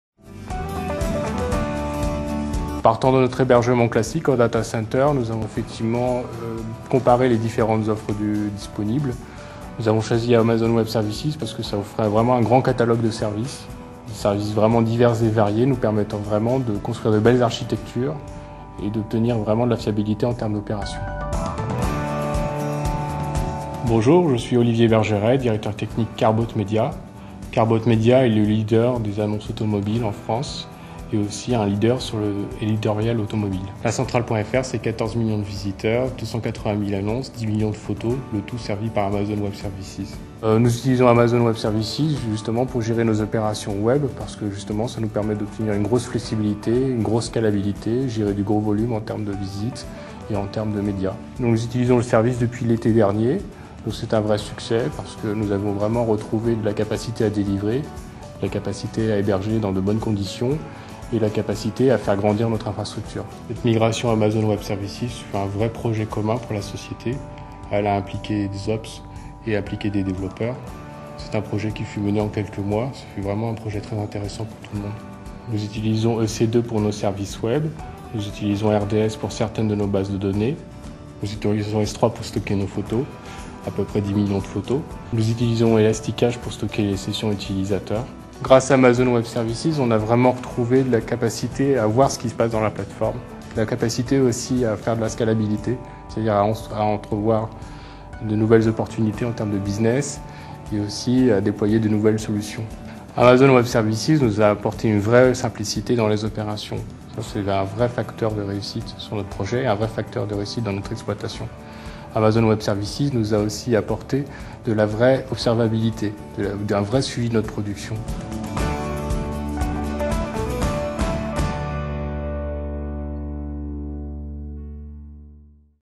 Car&Boat Media Uses AWS to Store and Serve Millions of Photos
Discover how Car&Boat Media, the company that runs France's largest online classifieds for used cars through uses AWS to serve 10 million photos to 14 million users each year.